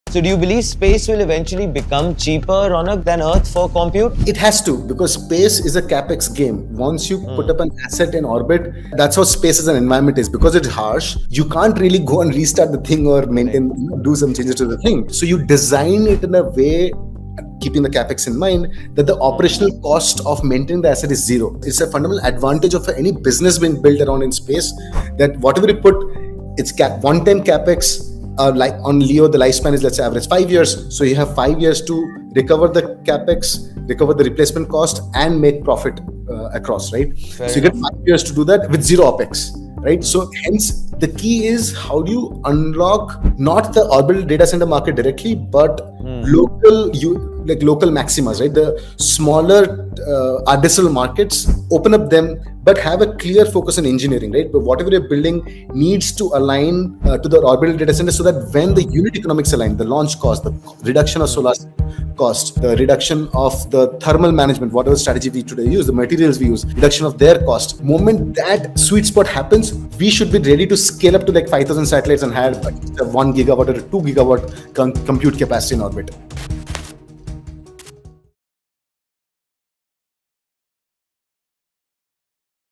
India's Space Data Center
Space must eventually become cheaper than Earth for compute because space is a capital expenditure (CAPEX) game. Since the space environment is harsh, and it's not easy to go and restart, maintain, or make changes to an asset in orbit, the design of the asset must keep CAPEX in mind.
The fundamental advantage of any business built around space is that the operational cost (OPEX) of maintaining the asset is zero.